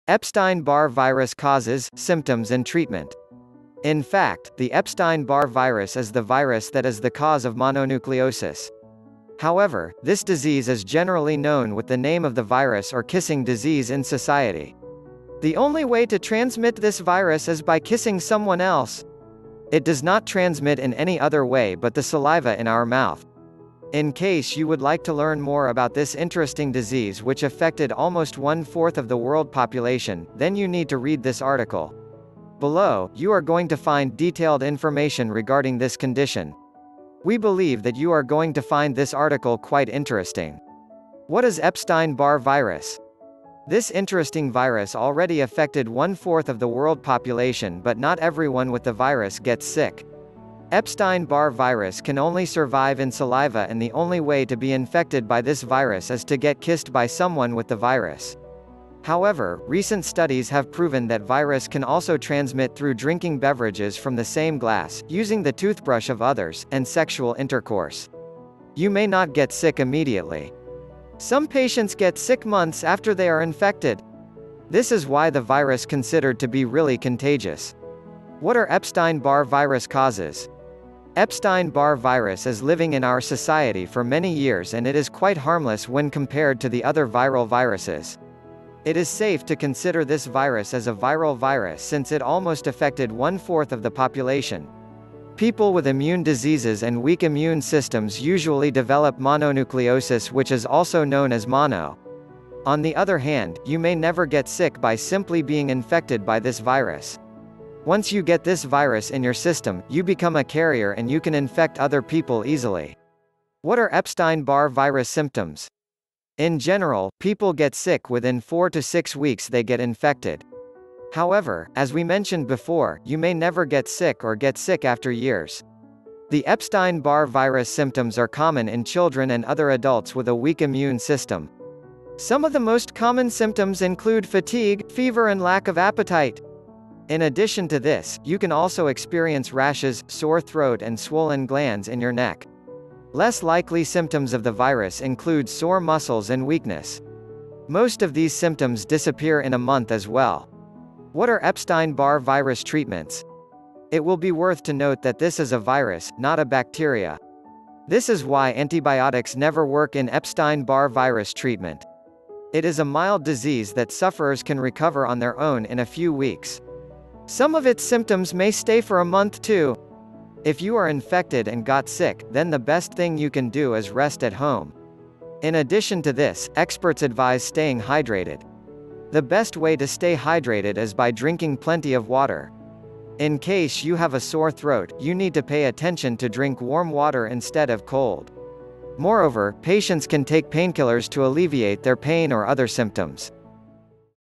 Epstein Barr Virus : Causes, Symptoms and Treatments.
In fact, the Epstein-Barr Virus is the virus that is the cause of mononucleosis. However, this disease is generally known with the name of the virus or kissing disease in society. The only way to transmit this virus is by kissing someone else. It does not transmit in any other way but the saliva in our mouth. In case you would like to learn more about this interesting disease which affected almost one-fourth of the world population, then you need to read this article. Below, you are going to find detailed information regarding this condition. We believe that you are going to find this article quite interesting.

Please read article to learn more information about the Epstein-Barr Virus.